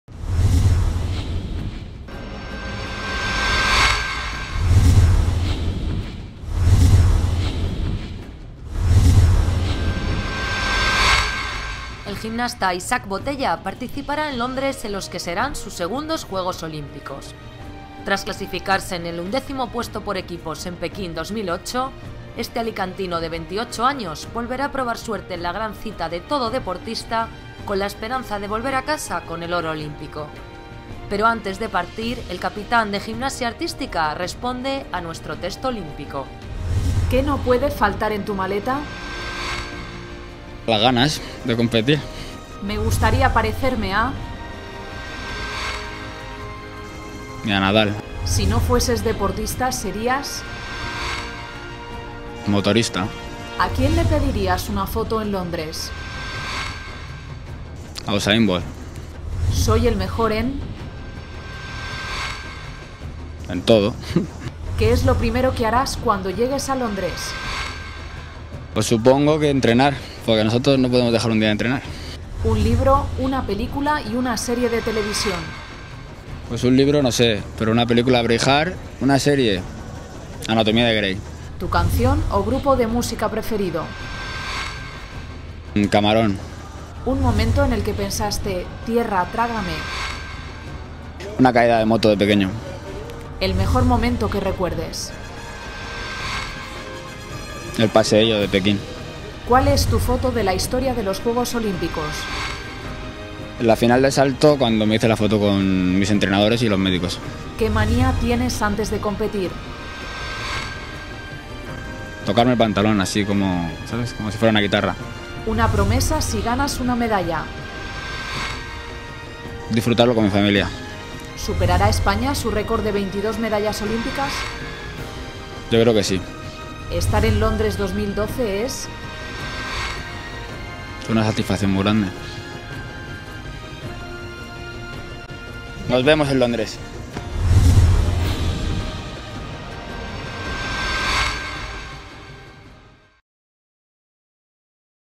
El test olímpico de Isaac Botella: "Si no fuese deportista sería motorista"
El capitán del equipo español de gimnasia artística, Isaac Botella, participará en Londres en los que serán sus segundos Juegos Olímpicos. Tras la undécima plaza conseguida en Pekín por el equipo de artística, el objetivo de este año es volver con la medalla de oro. Antes de partir, Isaac responde a nuestro test olímpico.